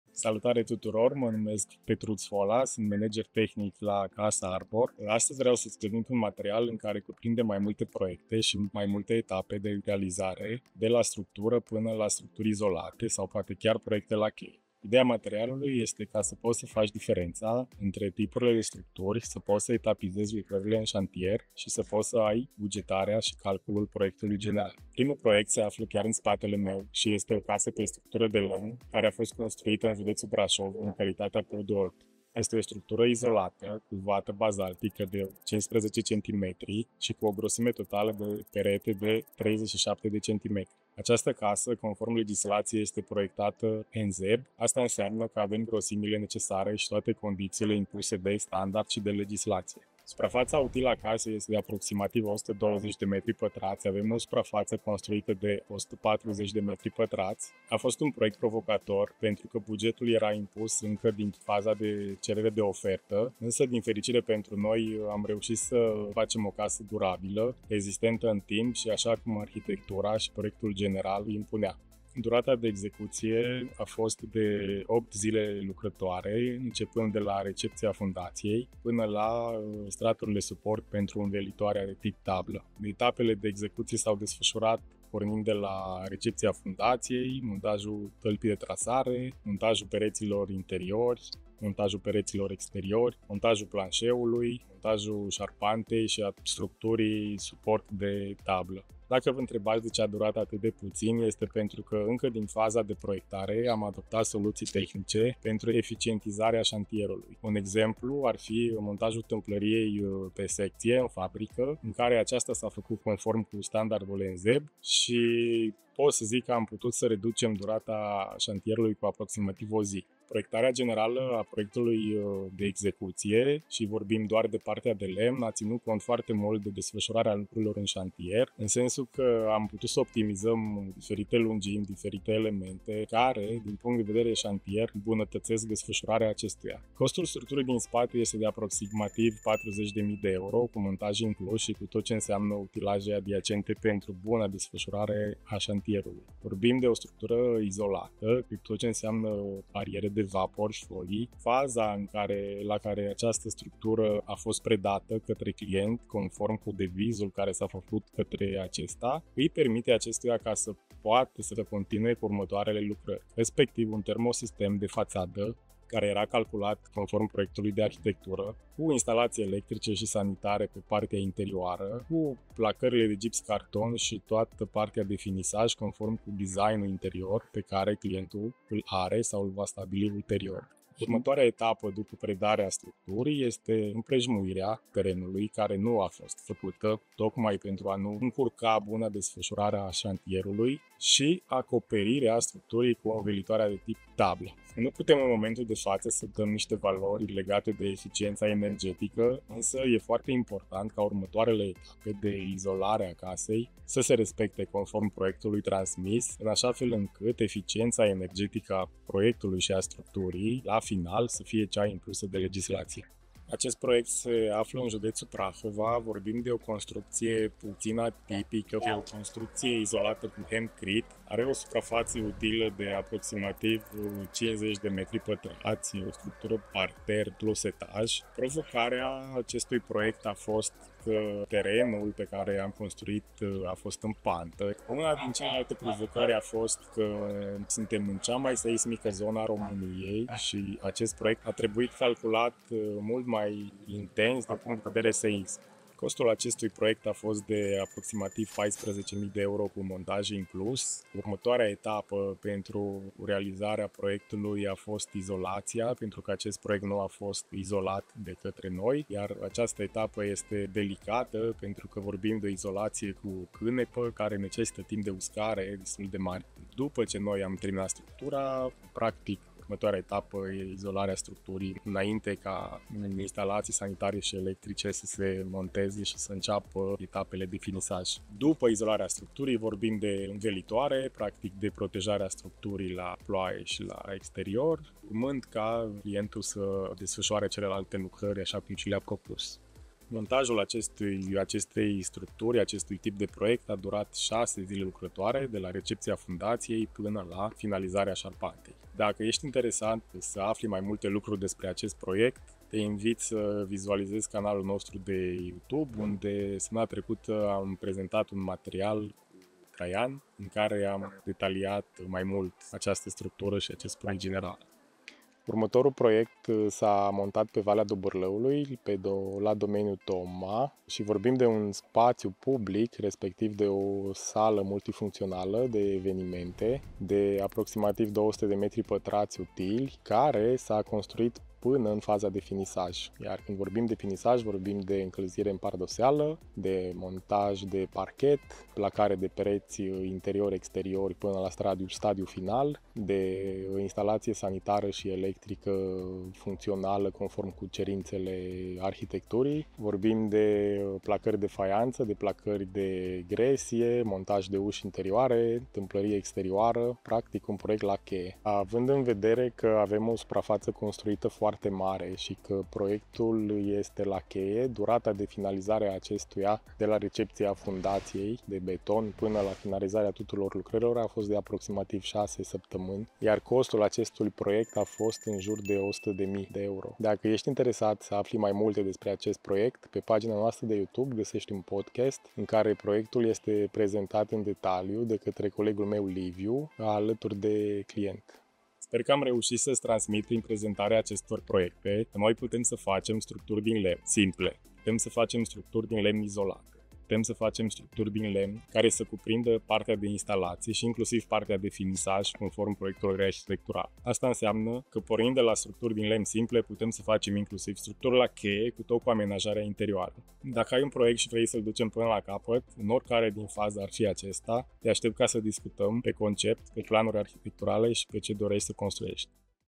De la structuri de lemn la proiecte la cheie - 3 construcții CASArbor
Petruț Fola, manager tehnic la Casa Arbor, te introduce în procesul detaliat al construcțiilor pe structură de lemn. De la case izolate în conformitate cu standardele nZEB la proiecte unice cu izolație de cânepă și spații publice la cheie, Petruț îți explică fiecare pas al execuției și inovațiile care stau la baza construcțiilor durabile. Vei descoperi cum optimizăm fiecare etapă pentru a scurta durata șantierului, cum alegem materialele potrivite, dar și cum ne adaptăm la provocările proiectelor în zone seismice sau pe terenuri dificile. Află cum transformăm planurile arhitecturale în realitate durabilă și personalizată pentru nevoile tale!

00:00 Prima construcție
3:33 A doua construcție
5:14 A treia construcție